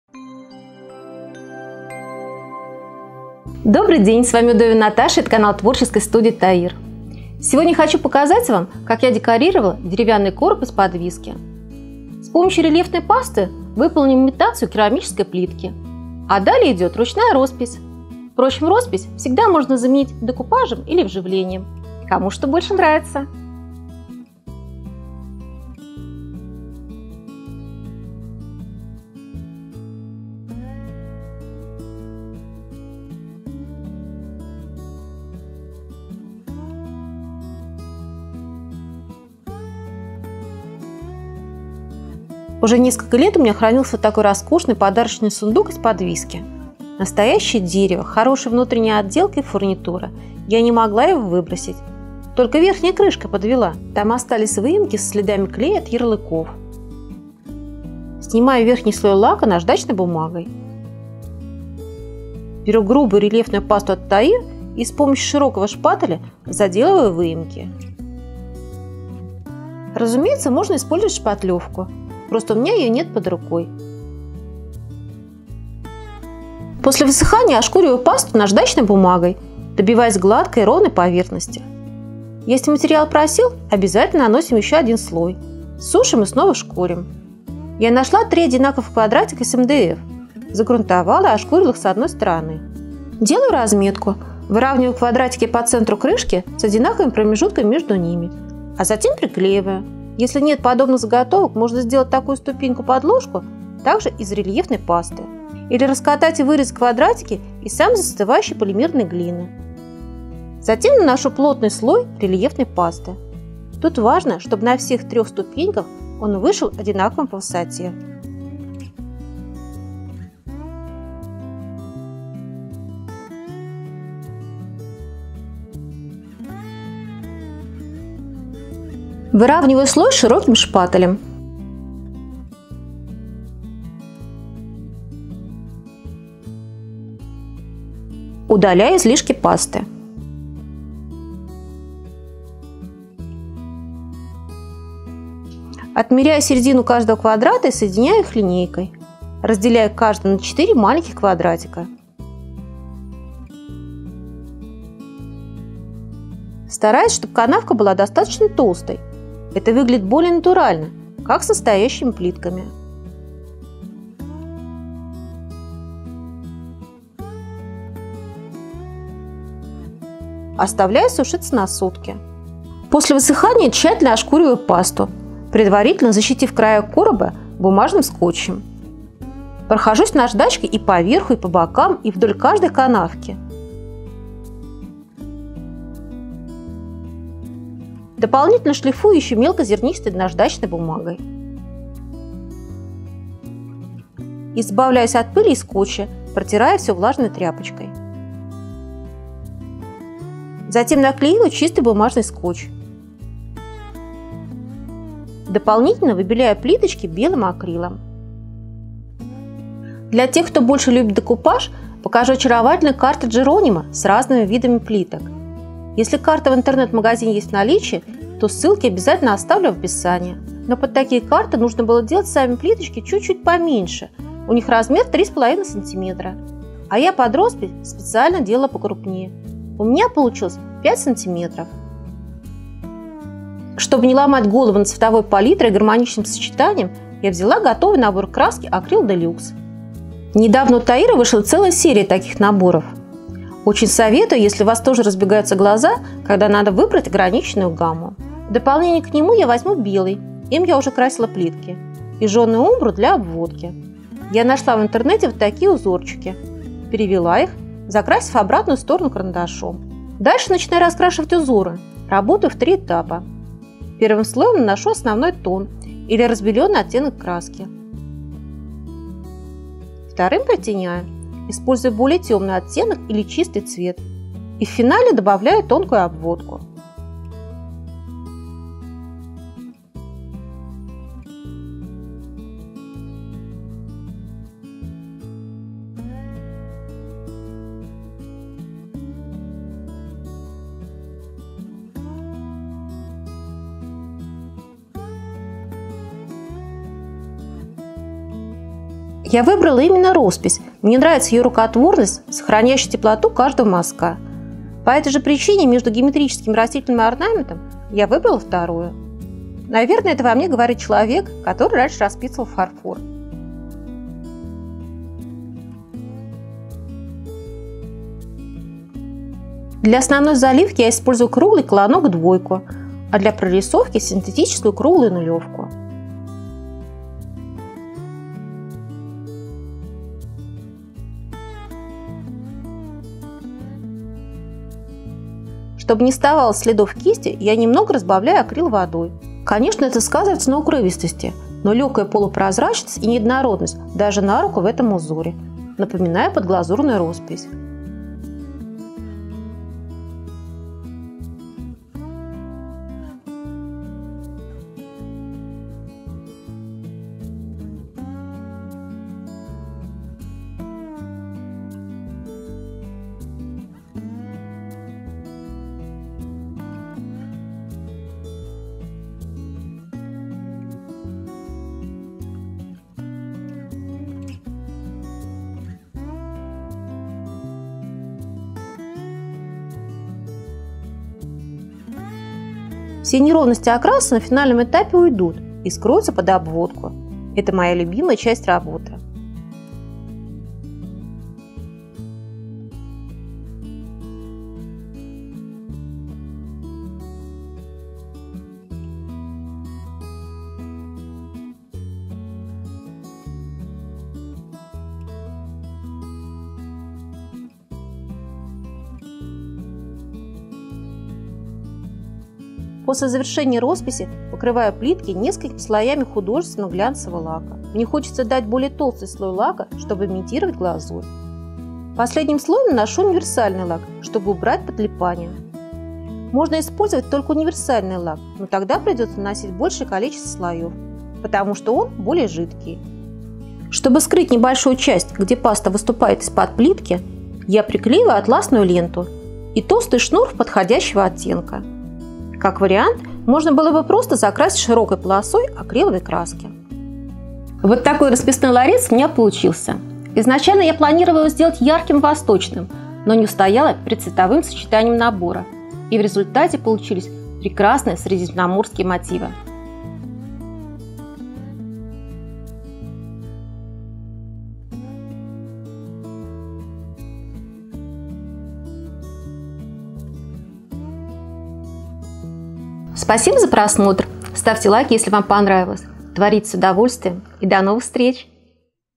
Имитация керамической плитки, рисуем узор в средиземноморском стиле. МК Наташи Удовиной
Стильный декор с расписной керамической плиткой можно выполнить даже не имея под рукой заготовок)
В этом видео мы покажем вам, как с помощью грубой рельефной пасты создать имитацию плиточек.

В качестве узора был выбран средиземноморский орнамент, вдохновение — новый набор от ТАИР «Рустик».

Ну и конечно, для тех, кто боится рисовать самостоятельно, покажем альтернативу)

• заготовки из фанеры квадратные;
• акриловая паста «Акрил-Арт»:
• наждачная бумага Р120 и Р400, мастихин, линейка, карандаш, шпатель
• кисти
- колонок круглая №2
- синтетика круглая №0

Также на сайте есть и другие варианты, если ввести в поисковую строку слово «Изразцы».

Продукция «ТАИР» на Беру: lnk.bio/s/e1C6/fXBs